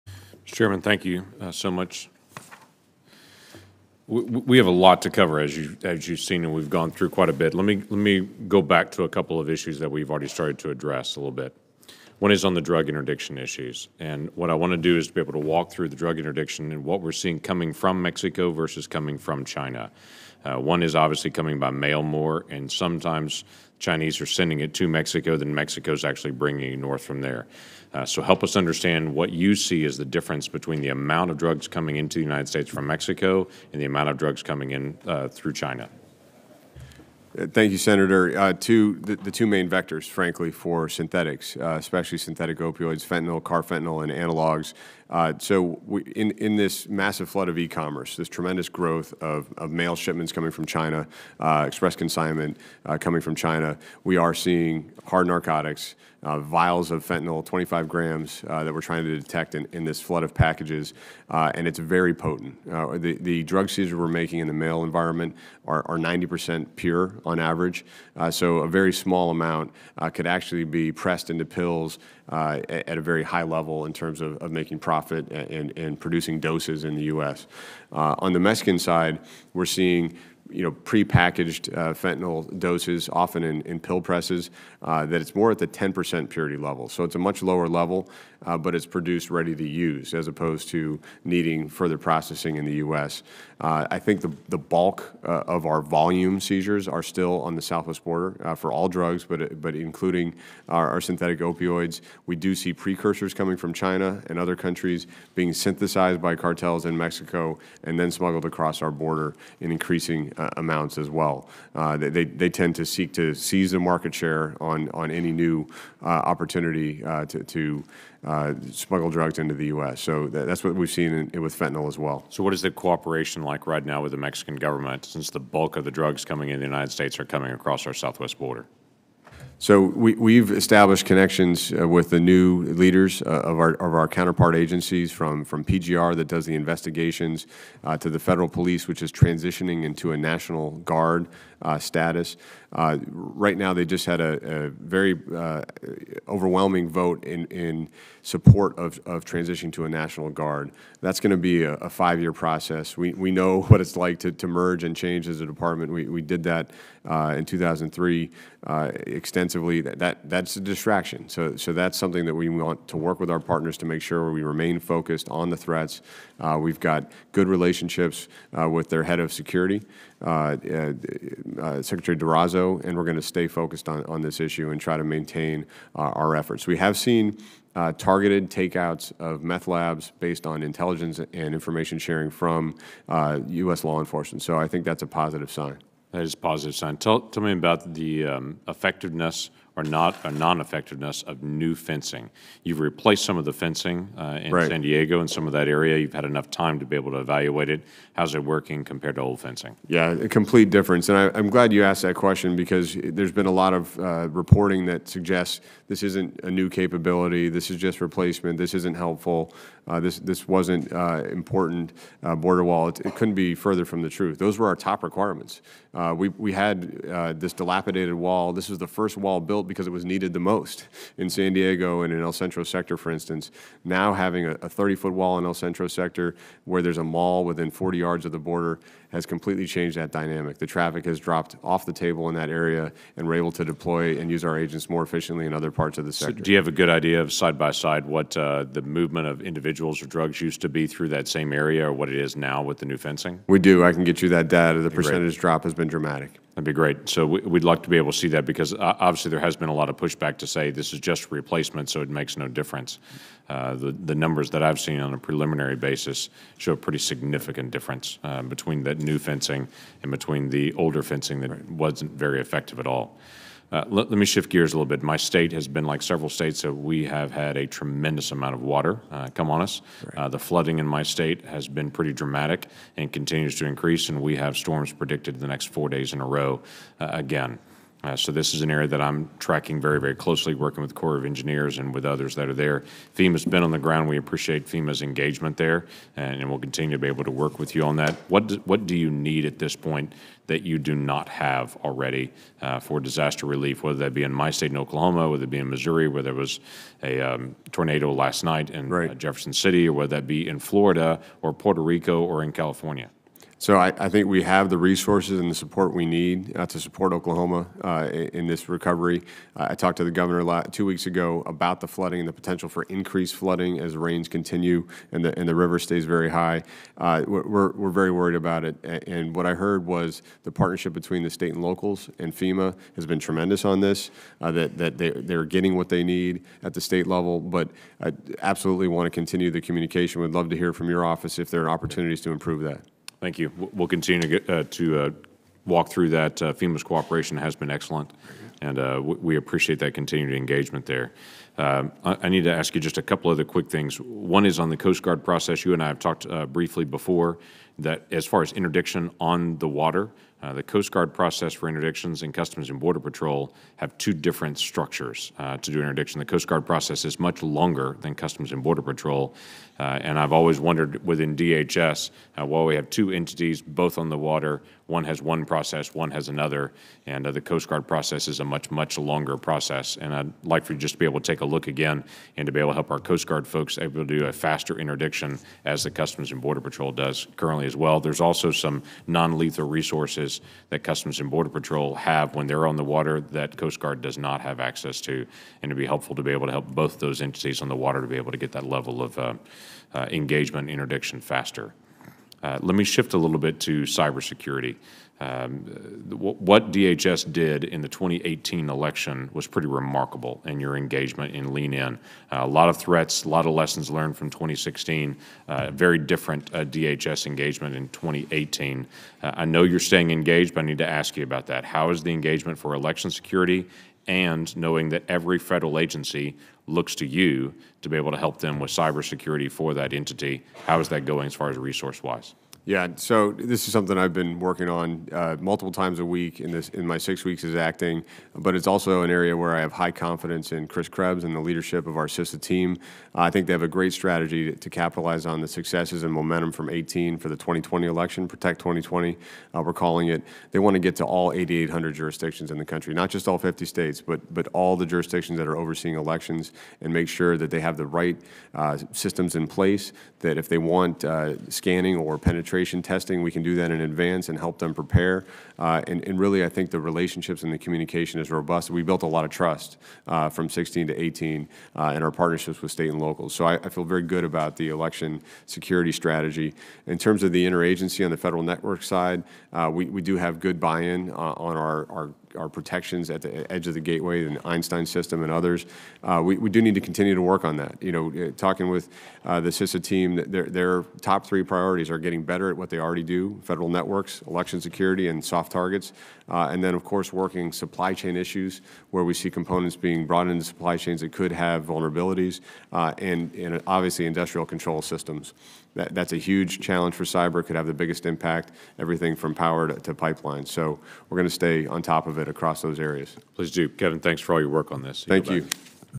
Senator Lankford Questions Acting DHS Secretary McAleenan
On May 23, 2019, Senator Lankford (R-OK) questioned the Acting Secretary of US Department of Homeland Security Kevin McAleenan in a hearing entitled, ‘Resources Needed to Protect and Secure the Homeland.’ Lankford asked McAleenan questions on drugs in the US from China and Mexico, the decreased movement of individuals with drugs due to new border fencing, and the current flooding in Oklahoma.